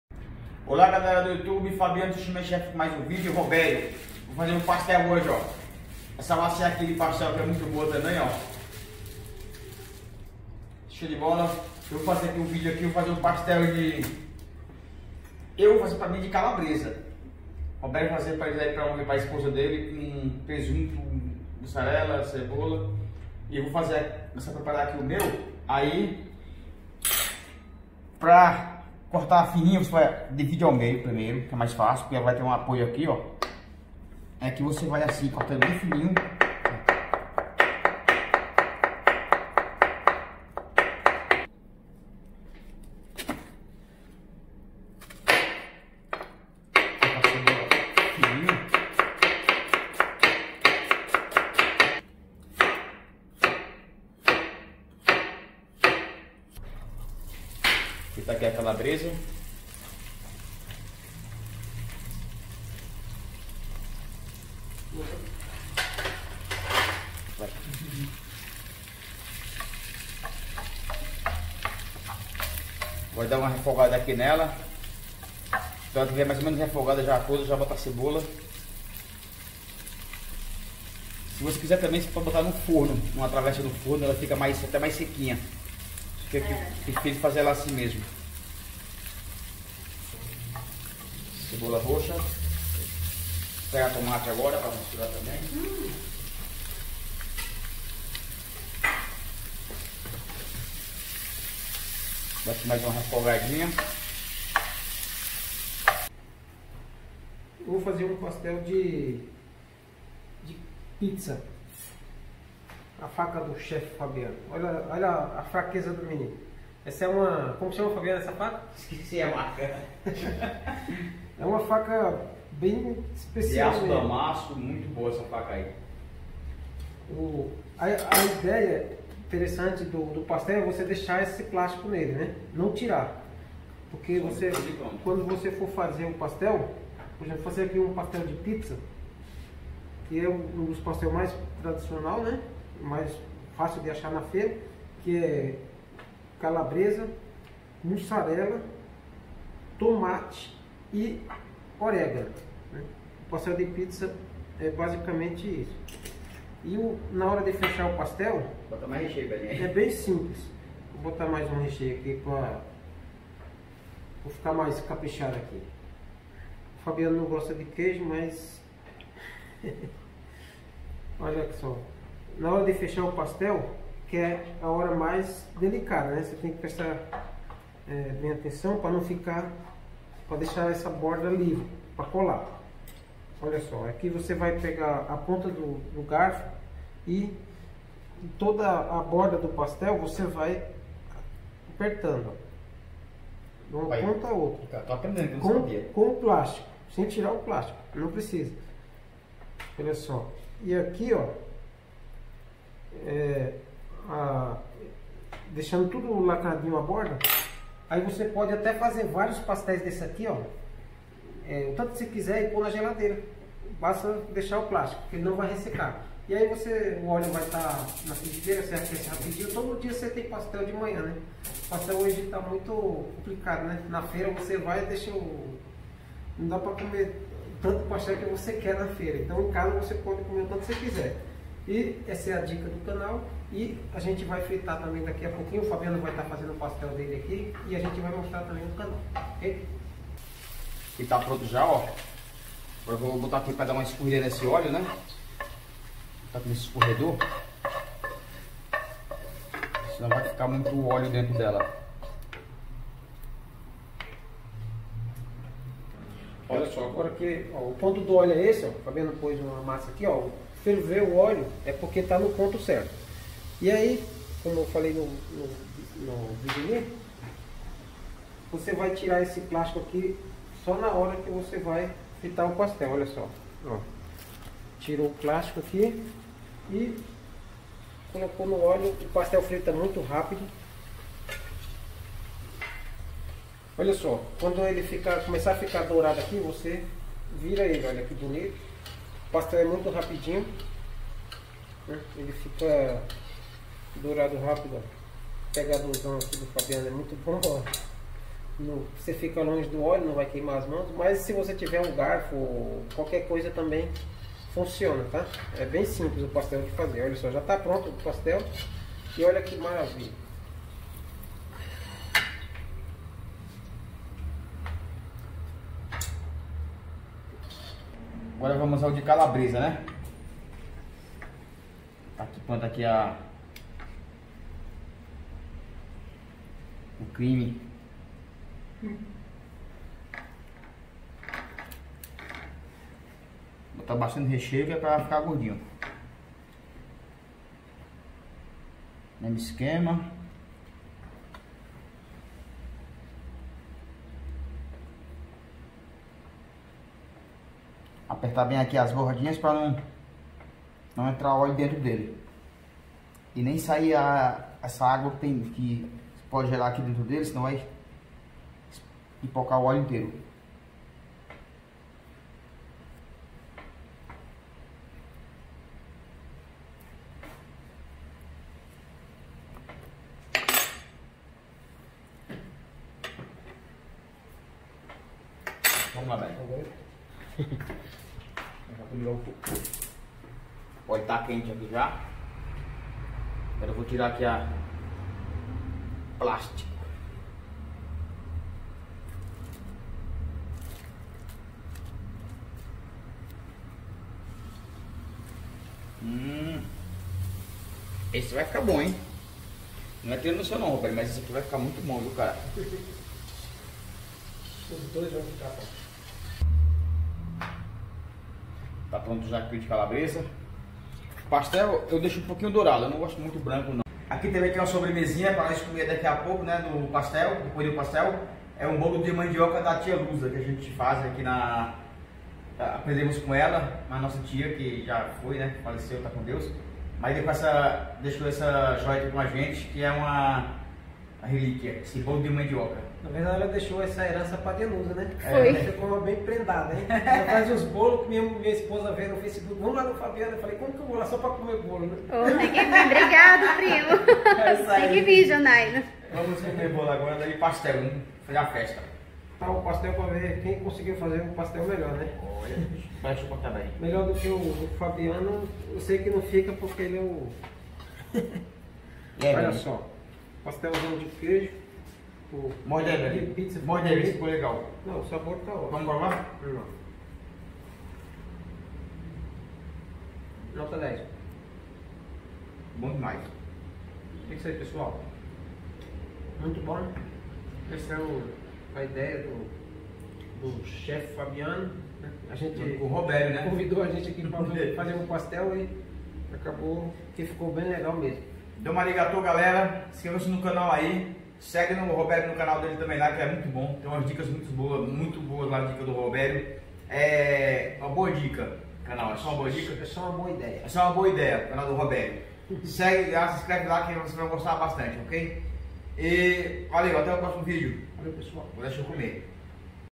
Pastel caseiro com vários recheios delicioso
Como fazer pastel Chef Fabiano com  Robério fazendo pastel
pastel caseiro
pastel caseiro
pastel caseiro